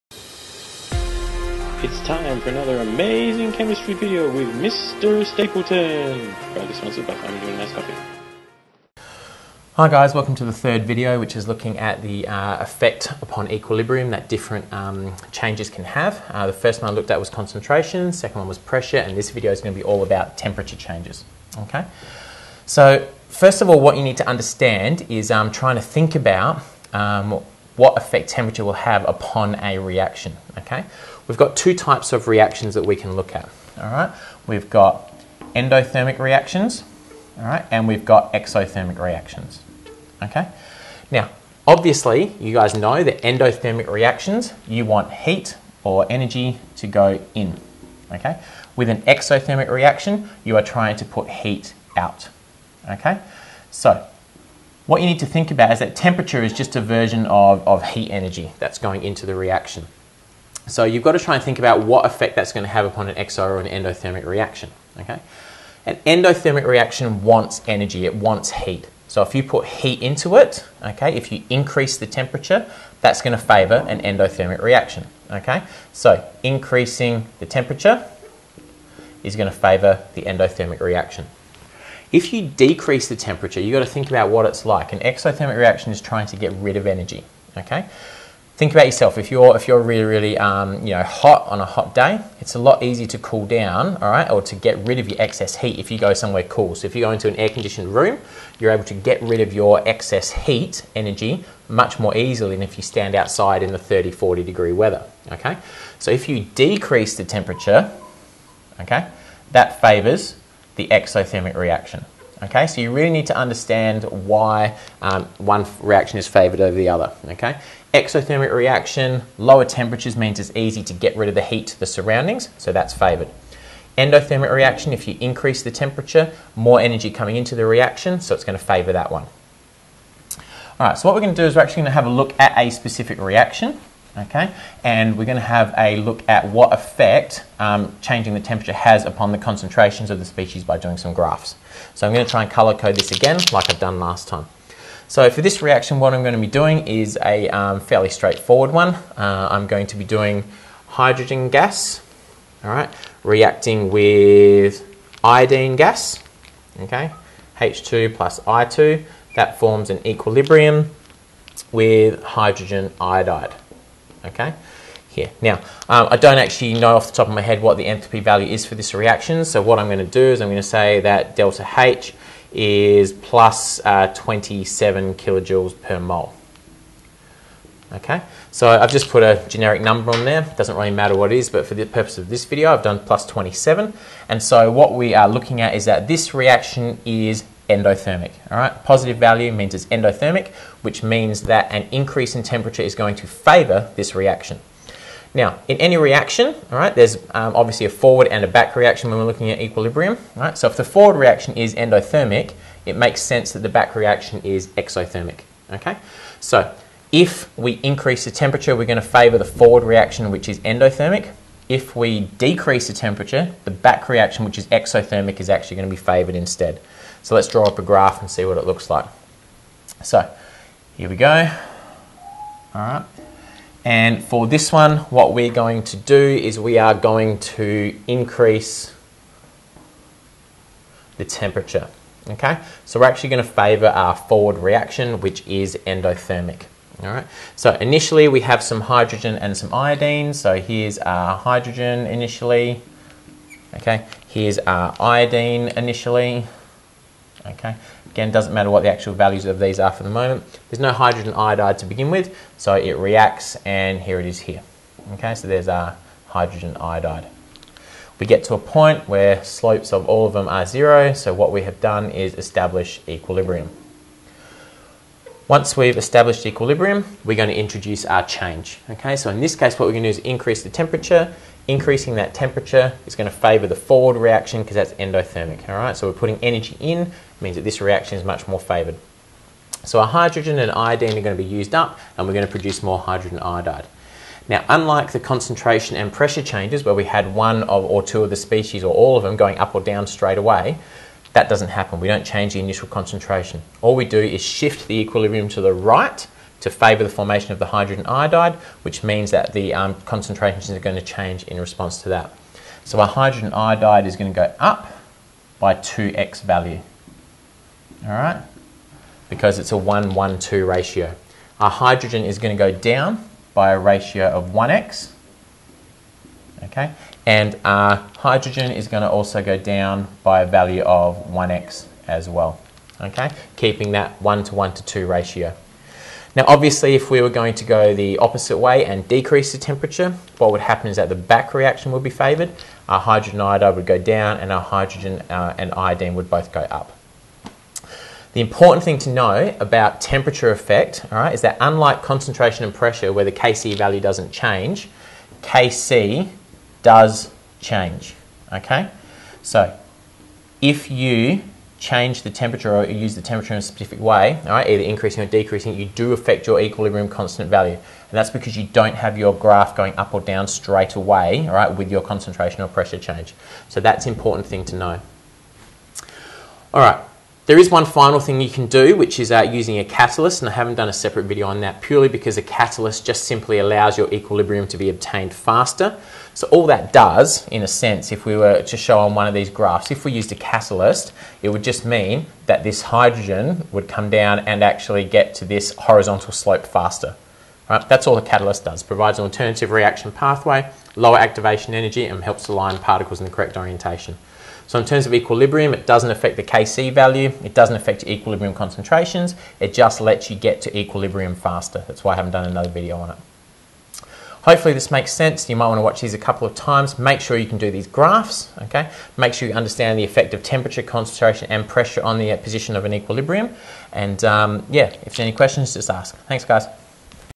Equilibrium #3 Effect of Temperature
Describes the effect of temperature on the position of an equilibrium. Make sure you watch the first two videos in this series first.

An increase in temperature favours the endothermic reaction, while a decrease in temperature favours the exothermic reaction.